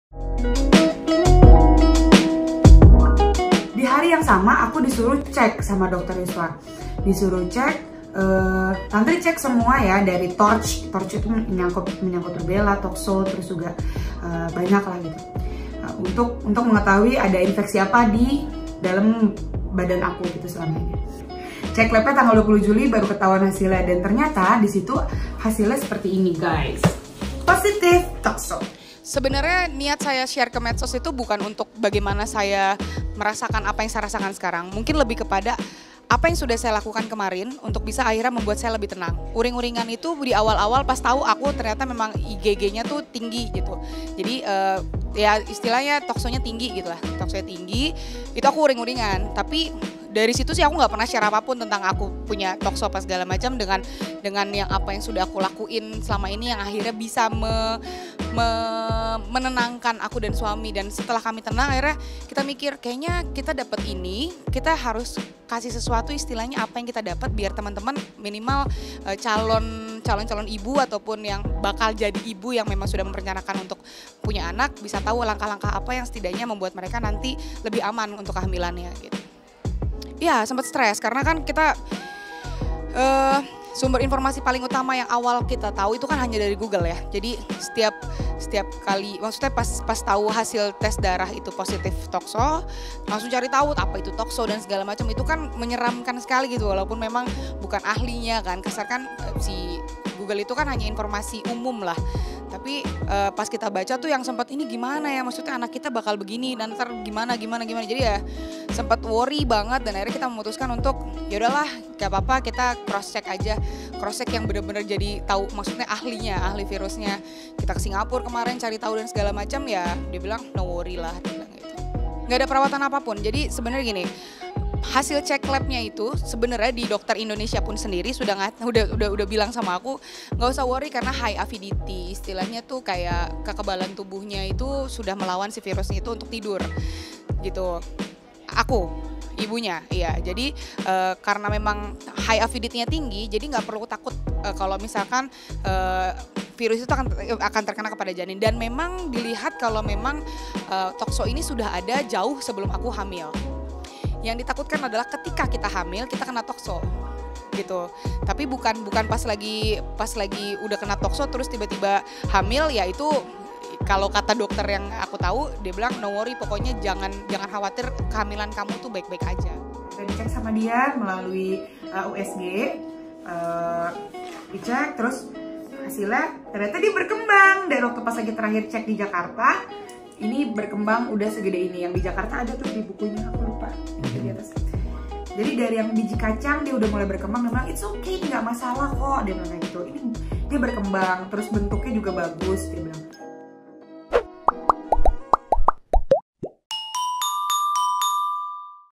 Niat Mulia Tantri 'Kotak' Curhat Hamil dengan Tokso di YouTube
Tidak ada niatan untuk mencari simpati netizen saat Tantri 'Kotak' curhat mengenai kondisi kehamilannya di YouTube. Maklum saja Bun, Tantri saat itu baru saja tahu bahwa dirinya terkena toksoplasma.

Menurut Tantri, niat yang ingin disampaikannya hanya memberi informasi kepada para calon orangtua. Dengan begitu, mereka bakal mendapat informasi yang tepat saat mengalami kondisi yang sama dengan Tantri.

Artikel terkait:

Reaksi Tantri 'Kotak' Saat Tahu Kena Toksoplasma Ketika Hamil